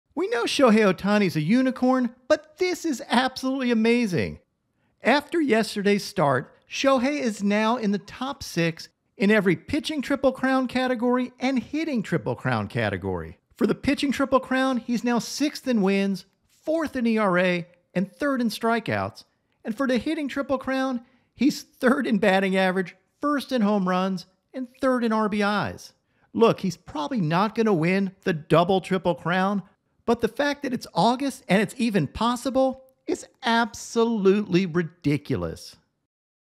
Hitting AND Pitching TRIPLE CROWN?? Shohei Ohtani is the GOAT
Shohei Ohtani is in the top 6 in all of the categories for both the hitting and pitching triple crown!